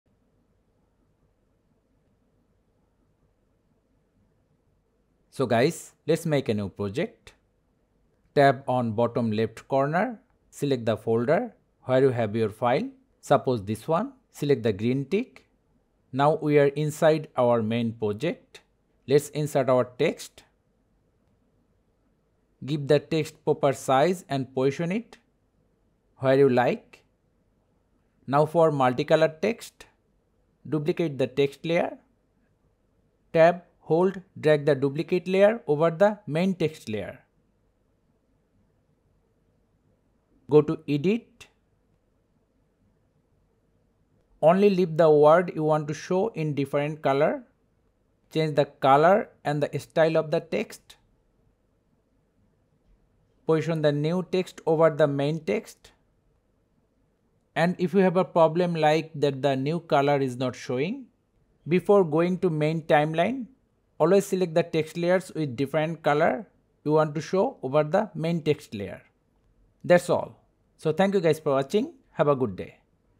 Multi-Color Subtitles, Captions, or Text | InShot Tutorial
In this tutorial, I’ll show you how to create a word-by-word, multi-color or colorful subtitle, caption, or text effect using "InShot" video editor. This is a beginner-friendly InShot video editing tutorial.